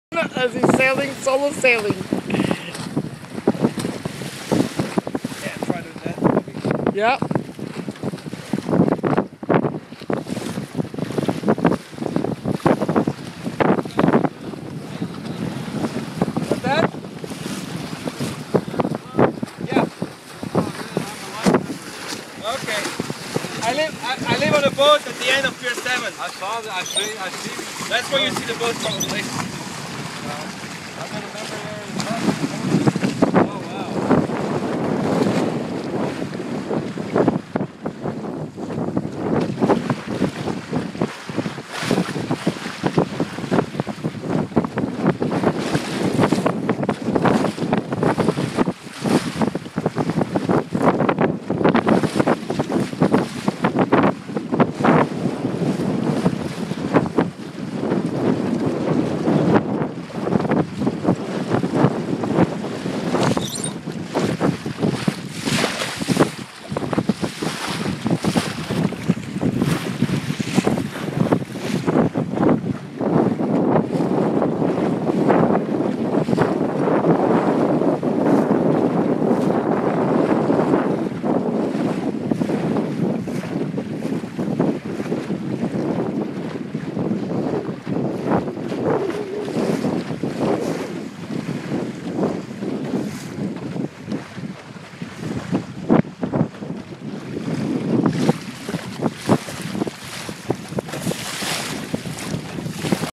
Sailing by a Lightning "Hi Voltage"
"Hi Voltage" sailing under beautiful sunset skies in Biscayne Bay, Miami, Florida.